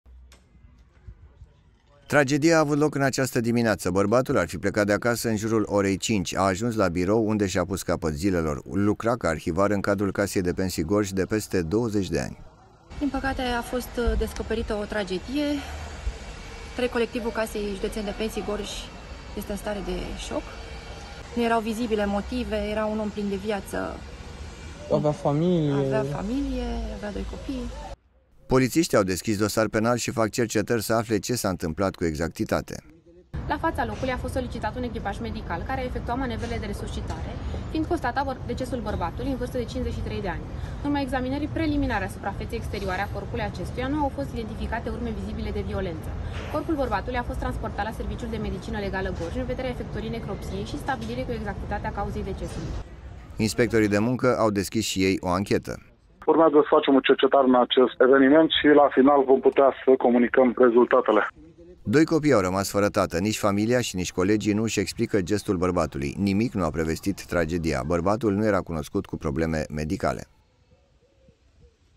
TRAGEDIE LA CASA DE PENSII GORJ
Un angajat al Casei de Pensii Gorj și-a pus capăt zilelor chiar în biroul în care lucra. Bărbatul era arhivar în cadrul instituției și avea o vechime în domeniu de peste 20 de ani. Astăzi, acesta se afla în concediu. Colegii sunt în stare de șoc și spun că nimic nu prevestea această tragedie.

Tragedia a avut loc, în această dimineață. Bărbatul ar fi plecat de acasă in jurul orei 5. A ajuns la birou, unde și-a pus capăt zilelor. Lucra ca arhivar în cadrul Casei de Pensii Gorj de peste 20 de ani.

Polițiștii au deschis un dosar penal și fac cercetări să afle ce s-a întâmplat cu exactitate.

Inspectorii de muncă au deschis și ei o anchetă.

Doi copii au rămas fără tată. Nici familia și nici colegii nu își explică gestul bărbatului. Nimic nu a prevestit tragedia. Bărbatul nu era cunoscut cu probleme medicale.